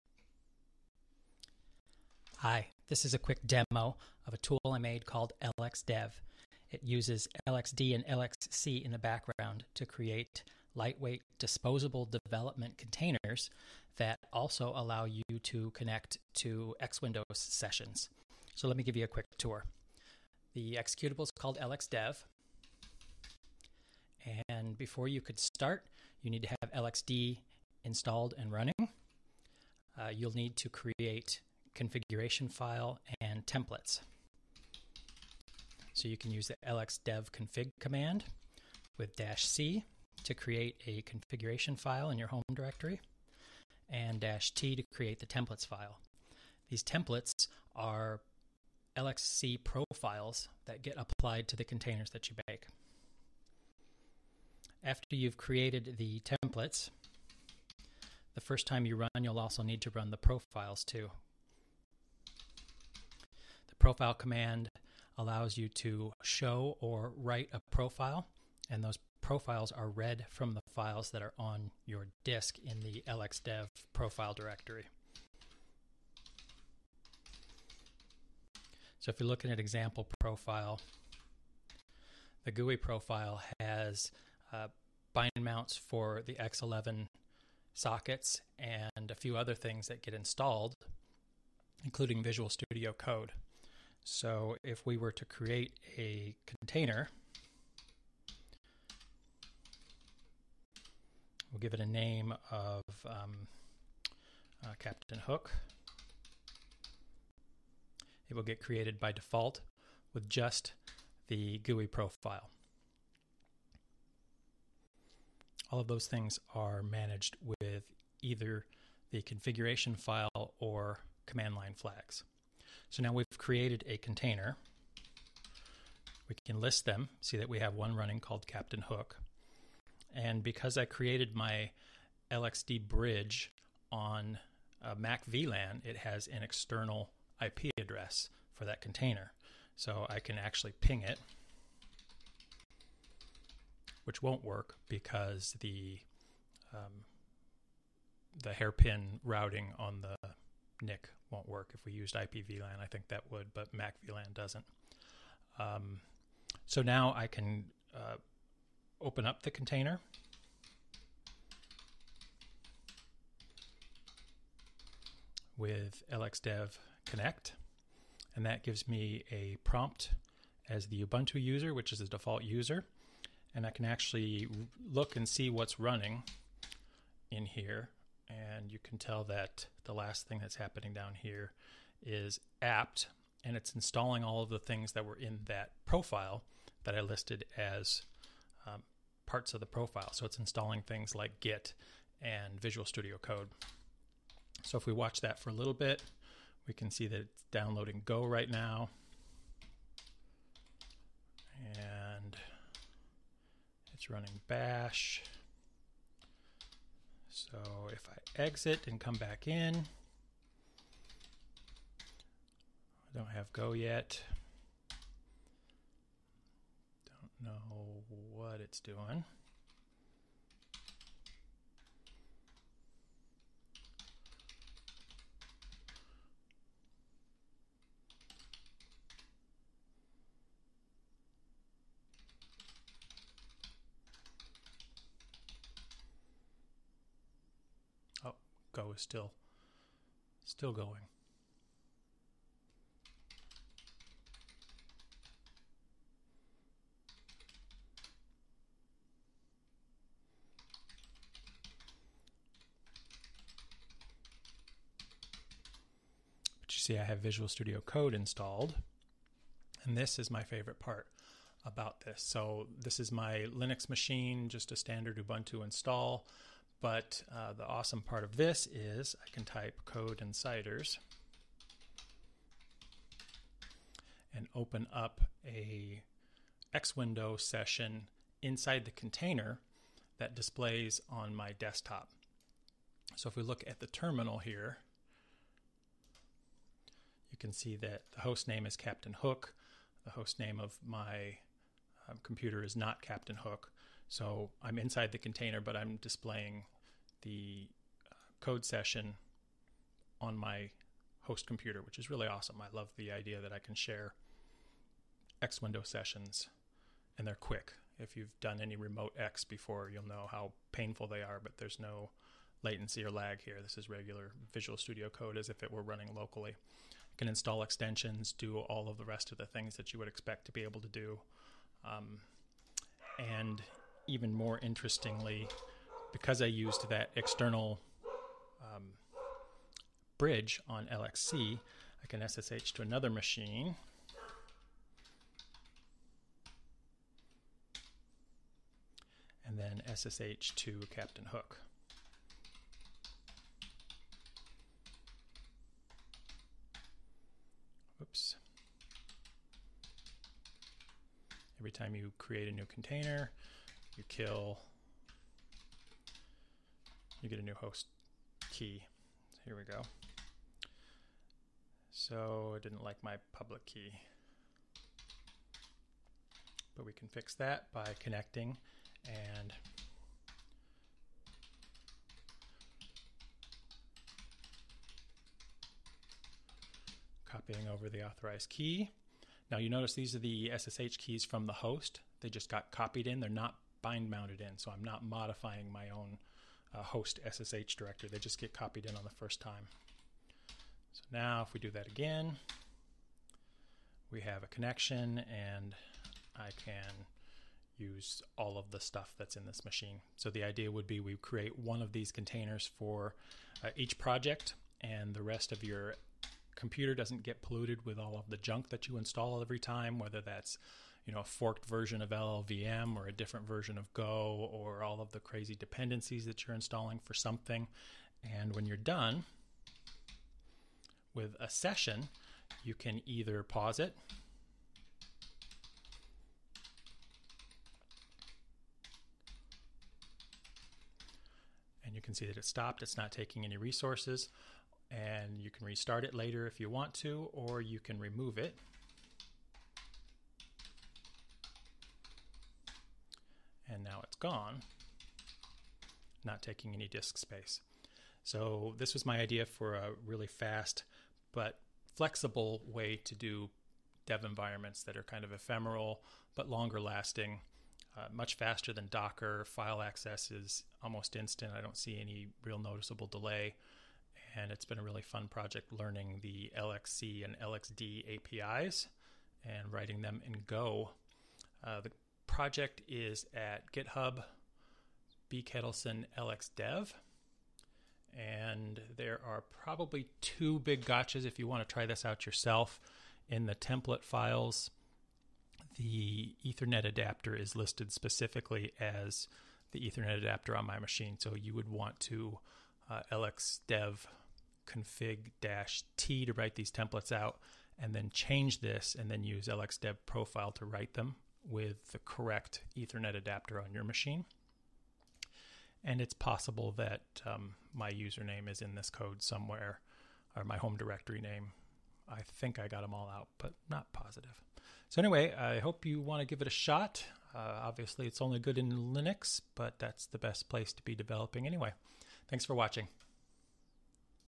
Demo of lxdev development environment tool (crazy high pitched audio?)
(Laugh at, or ignore the cartoon character high voice. Linux + Audio fail)
lxdev creates local development containers with full X11 and sound support using LXD/LXC on Linux.

Containers can be started and stopped as needed, and you can specify different profiles for each container, which will install extra tools (VS Code, Go, Slack, etc)